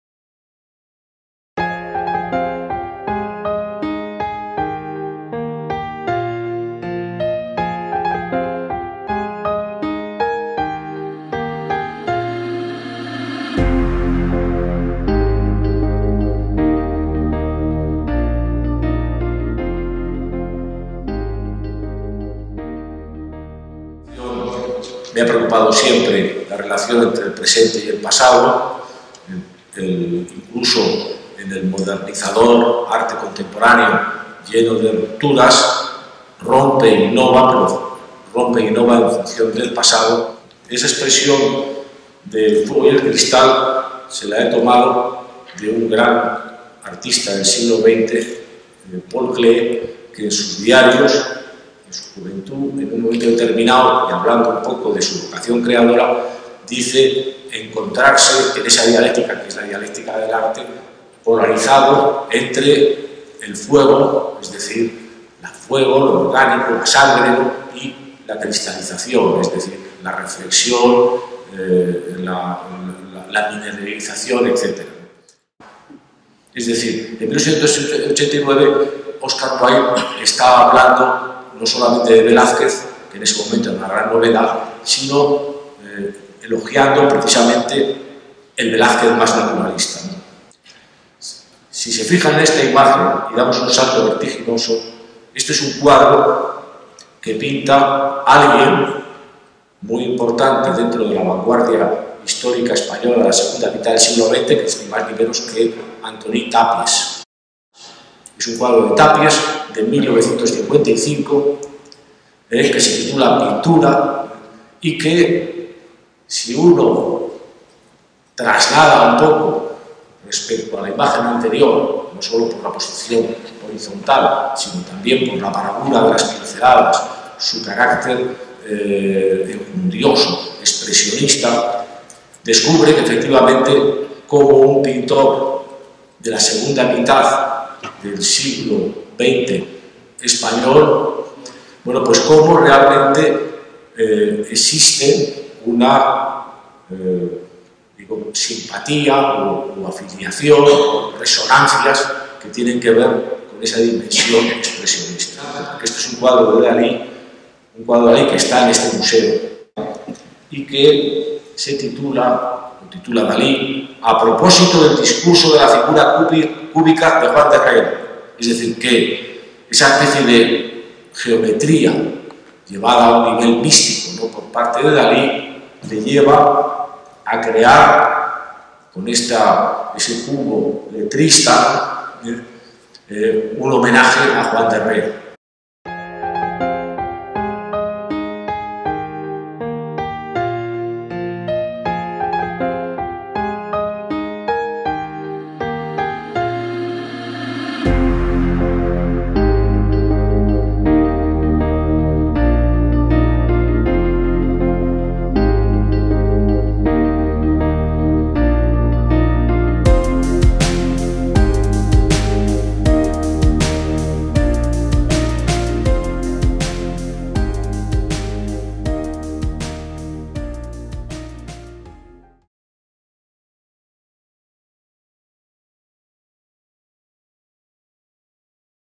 Conferencia inaugural del curso de Otoño
El arte español entre el fuego y el cristal
Francisco Calvo Serraller
Catedrático de Historia del Arte, Universidad Complutense de Madrid
Lunes, 19 de septiembre